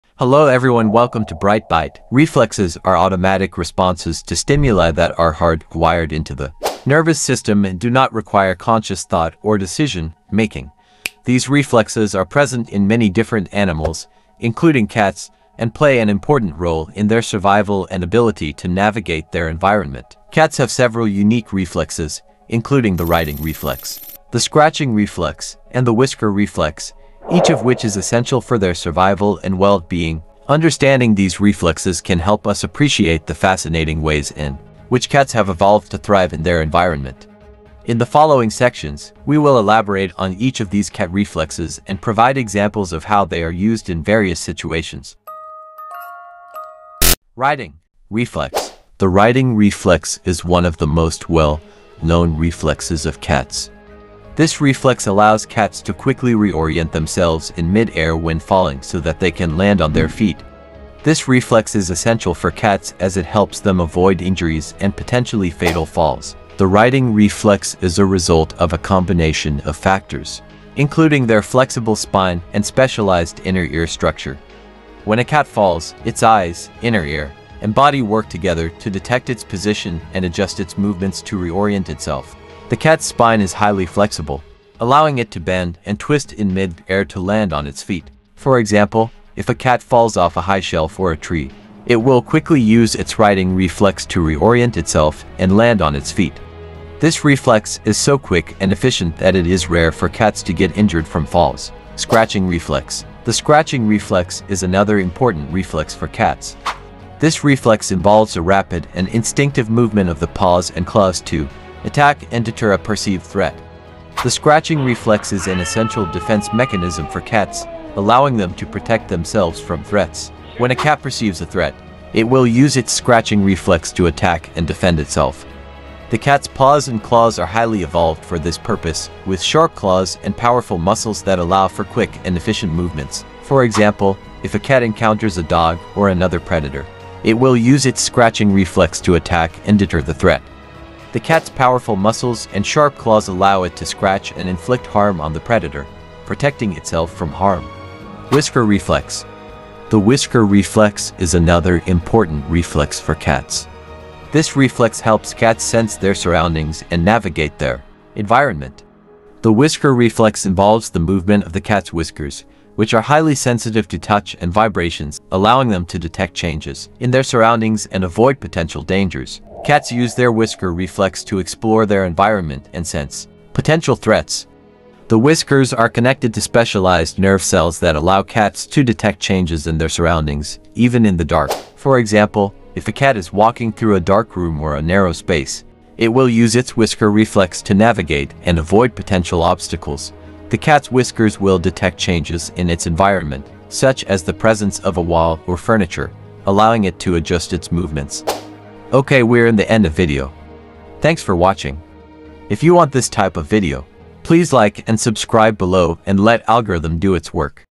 How Cats Defend Themselves: Exploring the Powerful Reflex🐈
How do cats manage to land on their feet? This is just one of the many incredible reflexes that these animals possess. In this video, we'll explore the fascinating world of feline instincts and delve into the unique reflexes that have helped cats to thrive in their environments. From the righting reflex that allows cats to reorient themselves mid-air, to the scratching reflex that serves as a potent defense mechanism, we'll examine the science behind these amazing abilities. Join us as we uncover the secrets of feline reflexes and gain a deeper understanding of these incredible creatures.